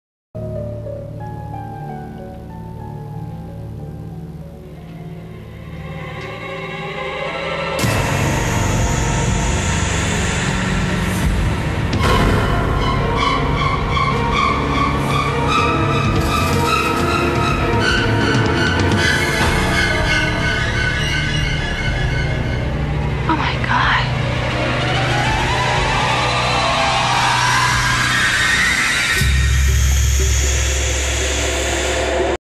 Victoria finds Gary's collage of photos
Victoria finds Gary's collage of photos on the wall in his apartment and finally figures out he is the stalker